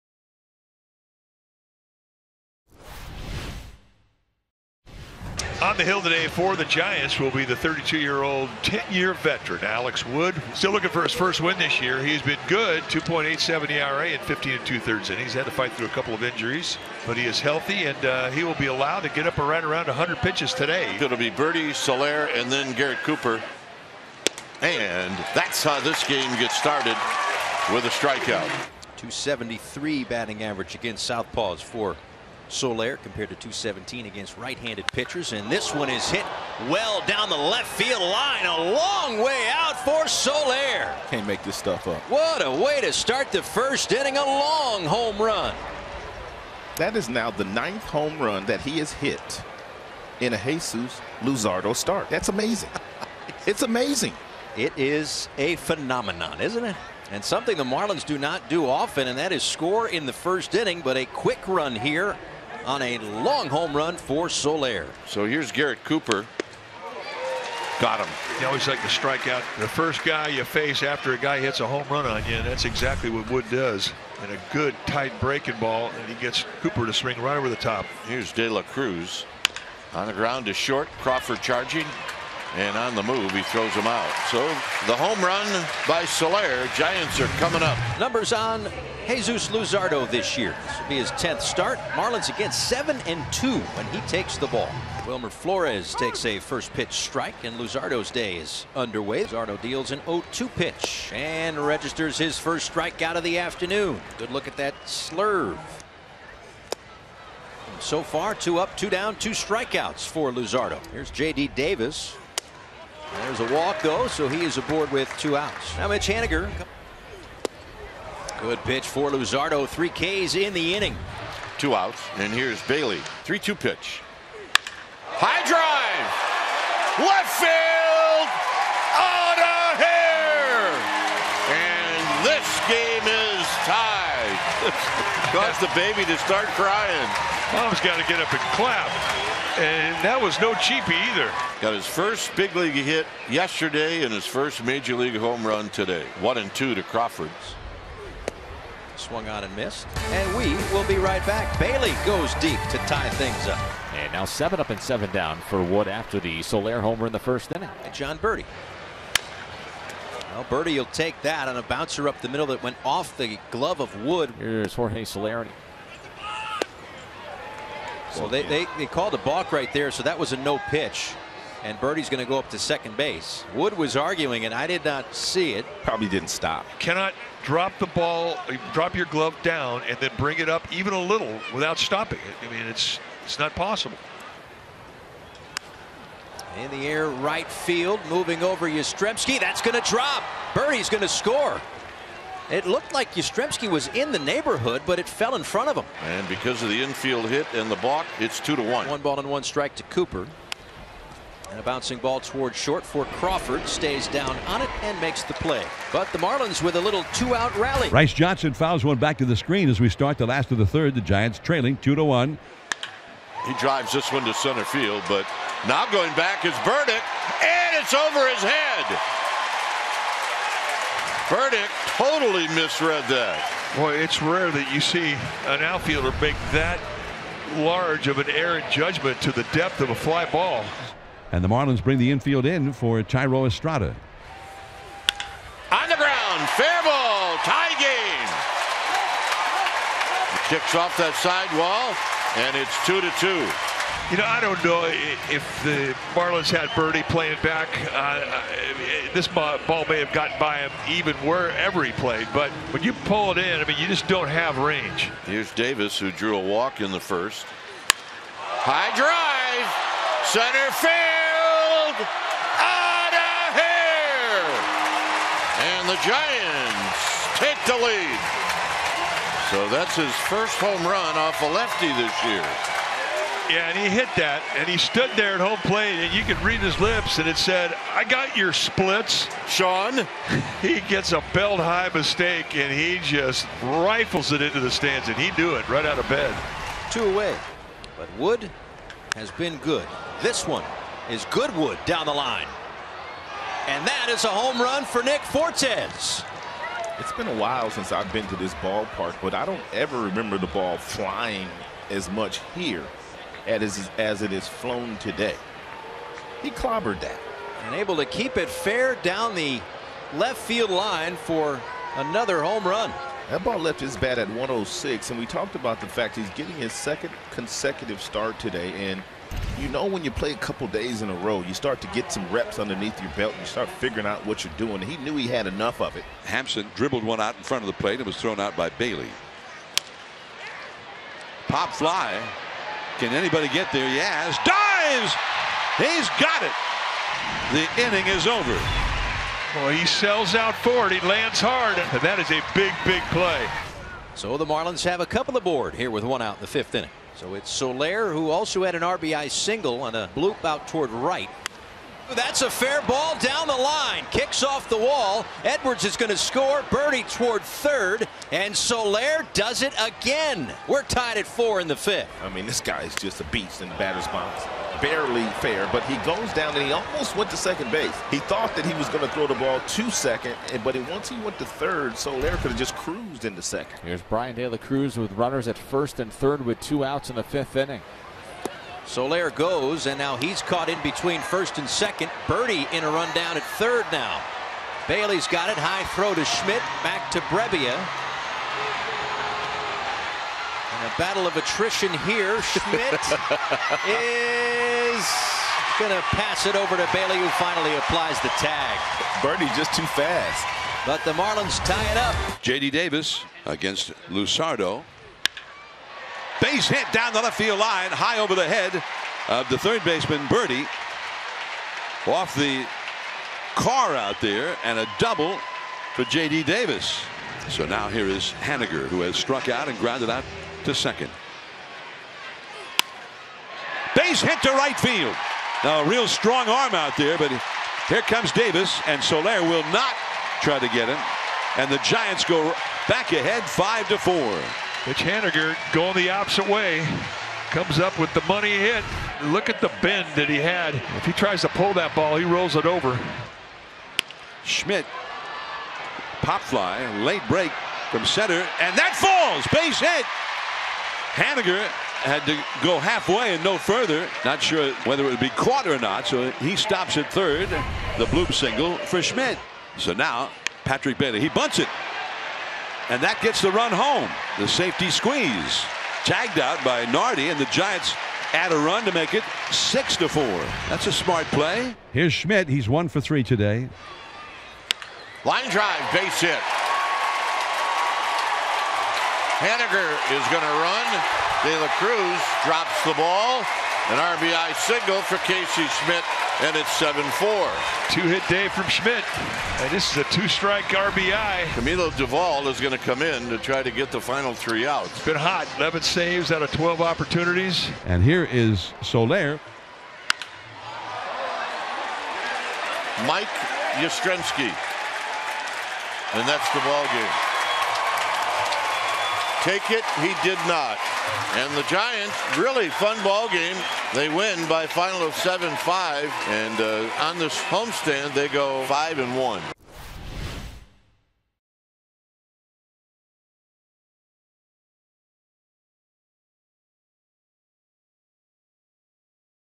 Marlins vs. Giants Game Highlights (5/21/23) | MLB Highlights
Marlins vs. Giants full game highlights from 5/21/23, presented by @WoodbridgeWines